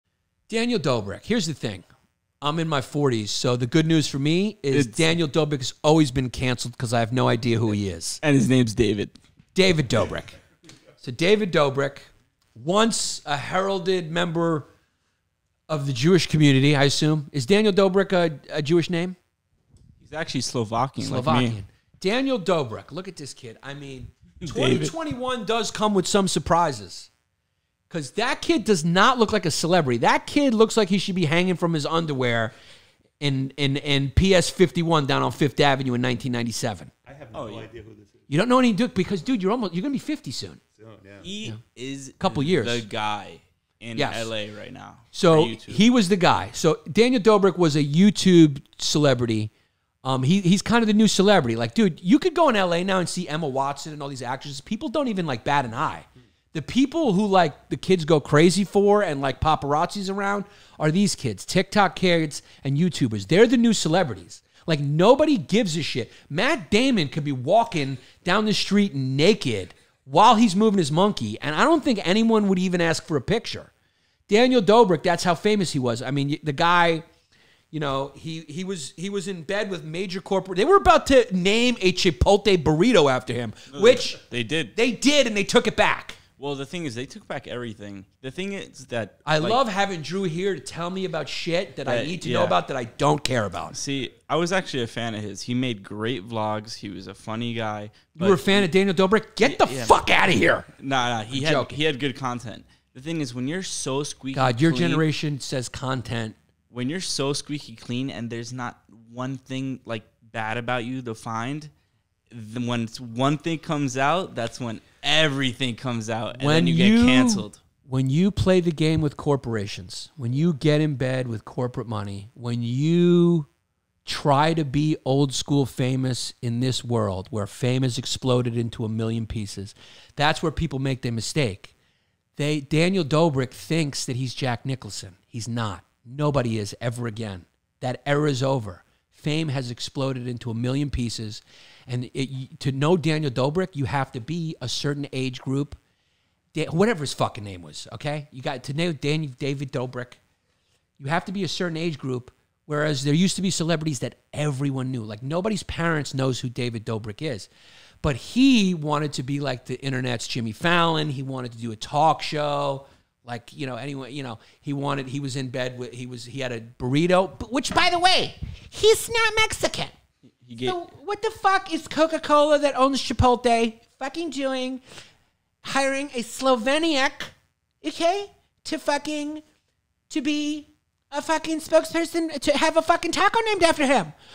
David Dobrik Is Cancelled | LongDays with Yannis Pappas
David Dobrik made a big, big mistake. Never get in he’d with corporations unless you got nothing to hide.